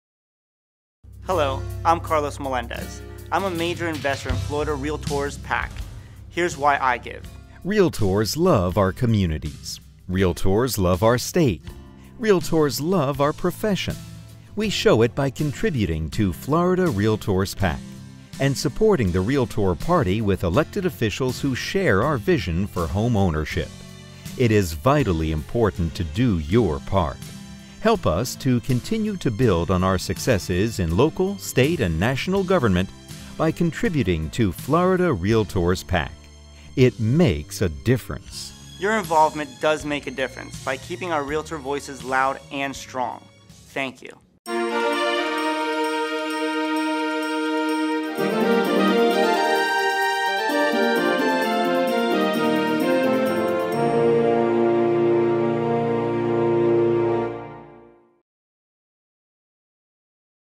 Why I Give to RPAC!
A Short yet informative video as to why i give to RPAC and you should too!